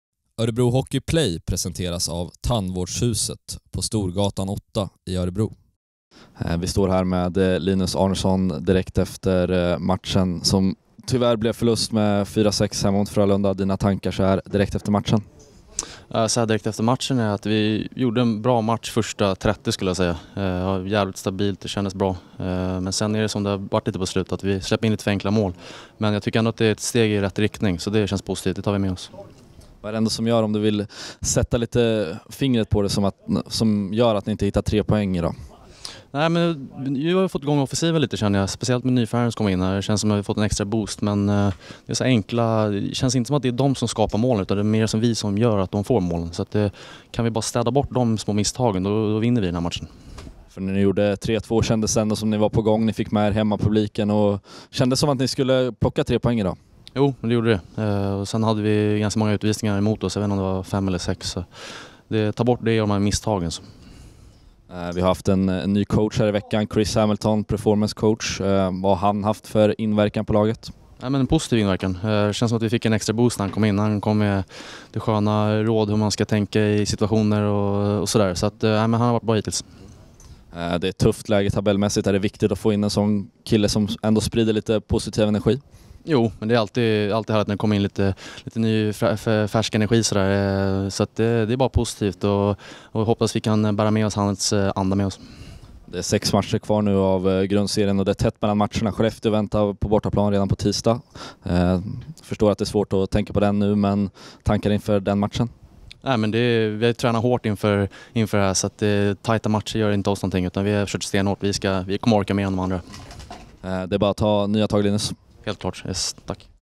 Linus Arnesson efter matchen mot Frölunda: "Gör en bra match första 30 min"
Linus Arnesson summerar lördagens förlust mot Frölunda i Behrn Arena.